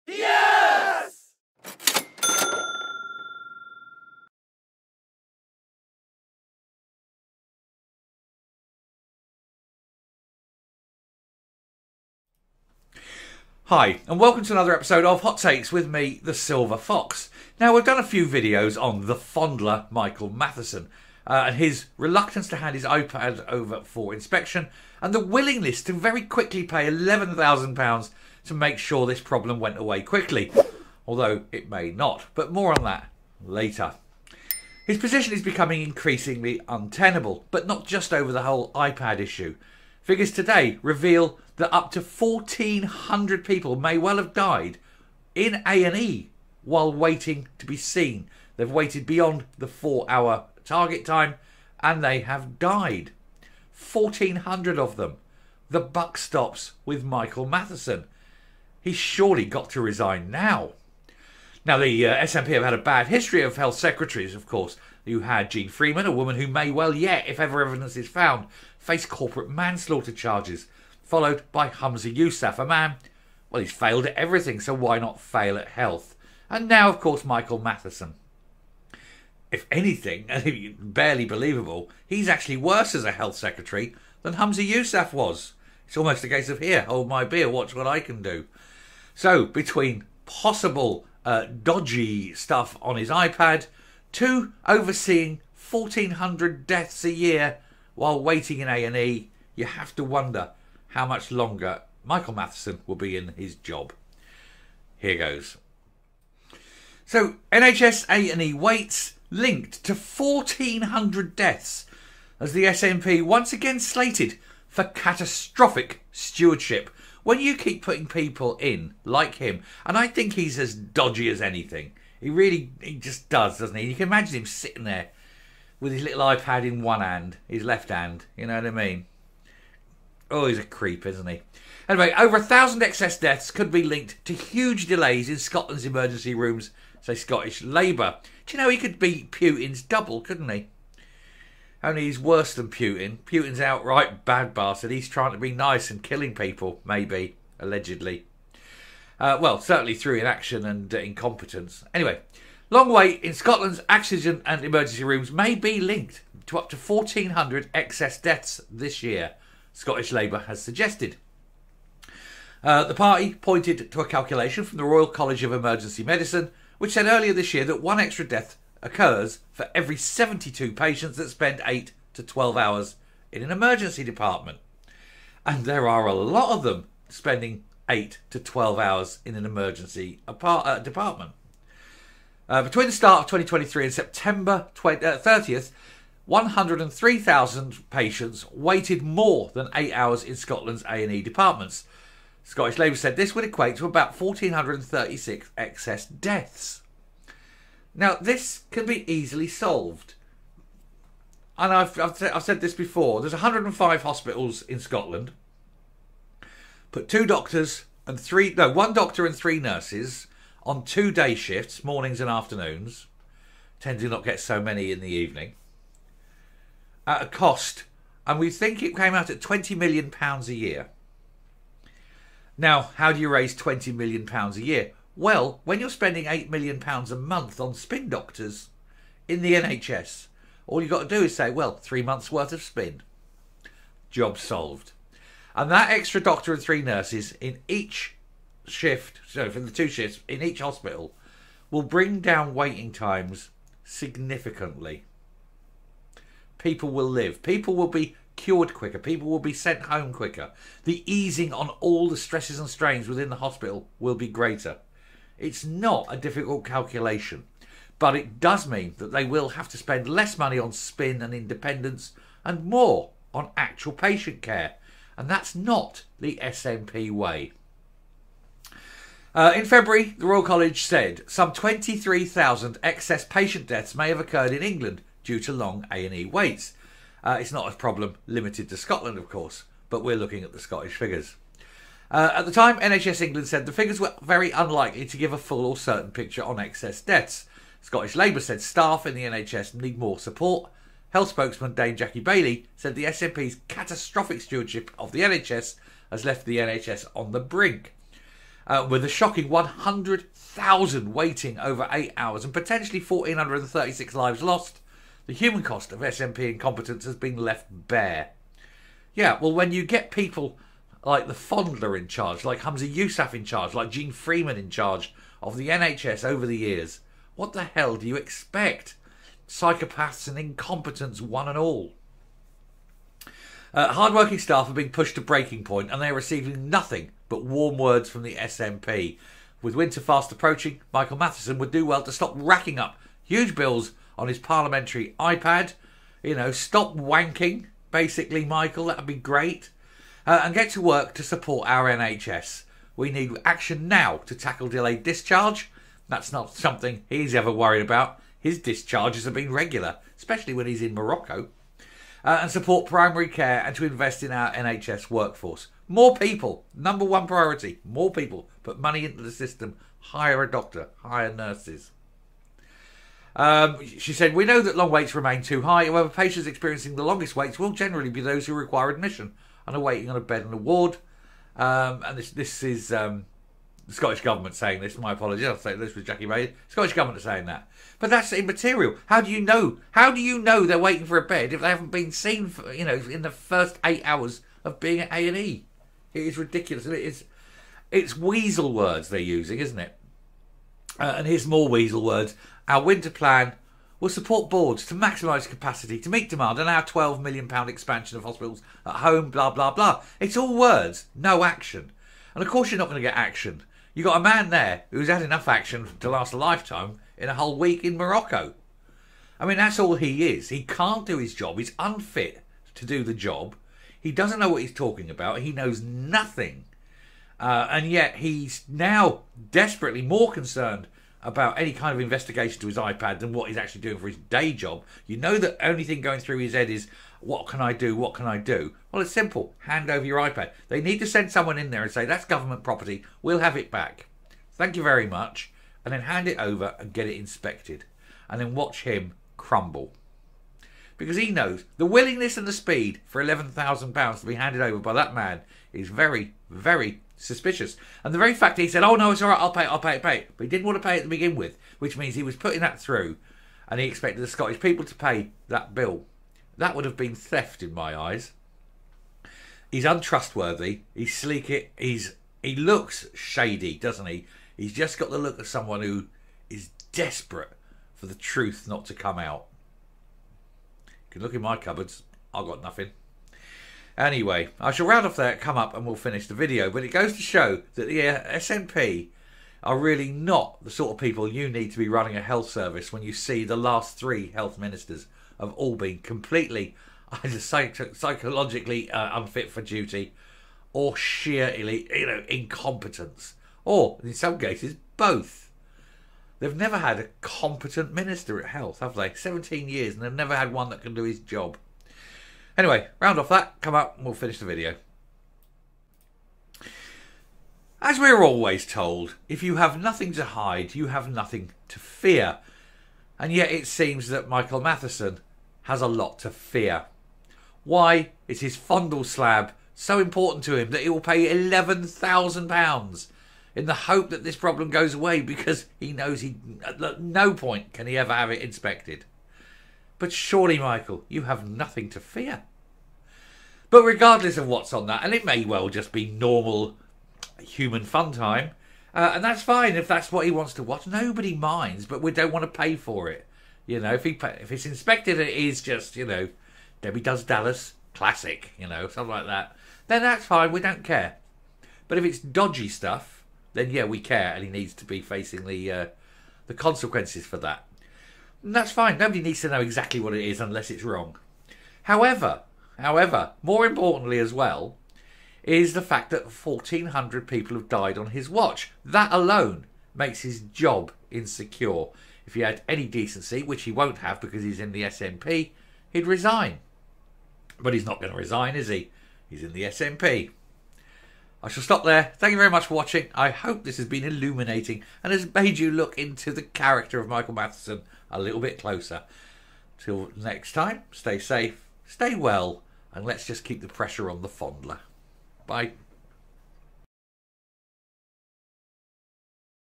A&E Waiting Times Kill 1400!
Michael 'The Fondler' Matheson needs to consider his position as over 1400 excess deaths have been blamed on long waiting times in A&E departments.
Meanwhile all he is concerned about is ensuring his iPad is not examined!